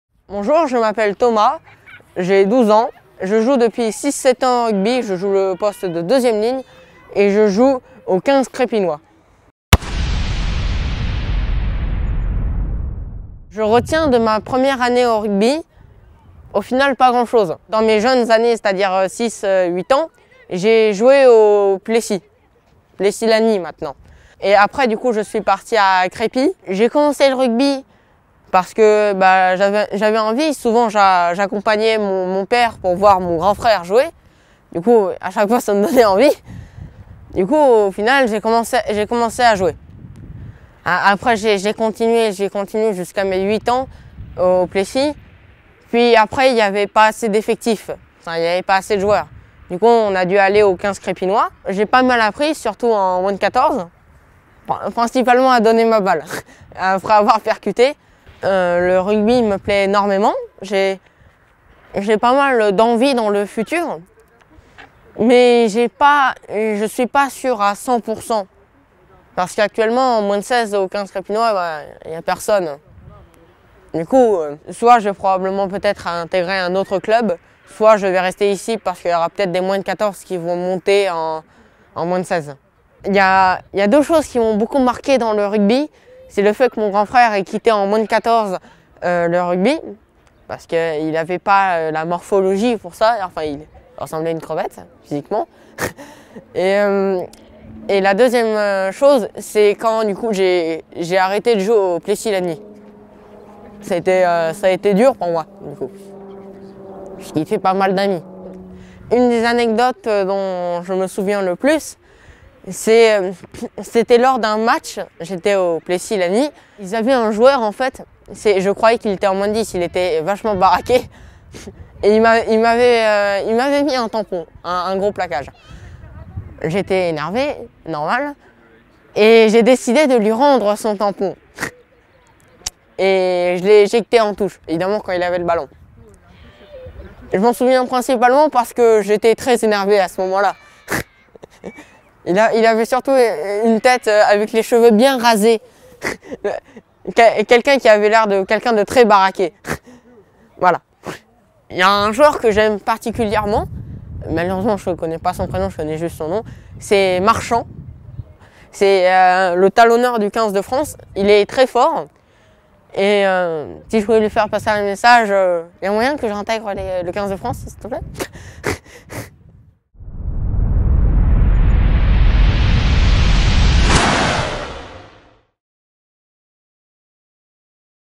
A Cœur Ovale - Épisode 12 : Thomas Bidon - 2ème ligne au XV Crépynois
Gare au vertige ! Nous voici revenus 30 ans en arrière ! Le jeune Thomas fort de ses 13 ans a néanmoins une expérience des terrains comme on les aime, puisqu'il a commencé au berceau (et encore, à l'époque il n'y avait pas "baby rugby" (en bon anglais dans le texte s'il vous plaît), ce qui permet aujourd'hui à ma nièce de 4 ans d'être entraînée "par le Père Noël" s'il vous plaît. Fin de la digression).

Entraîné quant à lui par Noël, un homme qui a joué toute sa vie et désormais transmet son savoir à Crépy-en-Valois, on est au sommet de la tradition ! Qu'on vienne pas me dire dans "le Sud" que "le Nord" n'est pas une terre de rugby (et pis de toute façon, une terre, ça se conquiert ! A coup de foies gras, de confits et d'armagnacs s'il le faut).

Notre jeune ami maîtrise déjà la passe après contact, comme j'aimerais pouvoir en dire autant de certains vieux briscards avec qui je me saou... enfin je joue. Même avant contact j'aimerais bien puisqu'on en est aux confessions intimes...

Pas suffisamment de joueurs en -16 pour monter une équipe ? Mais que fait la police ? Il est temps de créer des évangélistes du rugby à Crépy ! Chassez les jeunes en classe, dans la rue, partout !

Et pour ceux qui se cachent derrière leurs écrans, j'espère que ces vidéos leur donneront envie de venir se droguer au crampons et au gazon (la meilleure herbe qui soit, 100% naturelle, bio et locale. Et la boue, c'est cadeau).

Bon courage Thomas !

Et bon tournoi aux Bleus, on en bave d'impatience (même si on sait que Thomas est prêt à suppléer en cas de blessure, c'est rassurant. Je suppose...).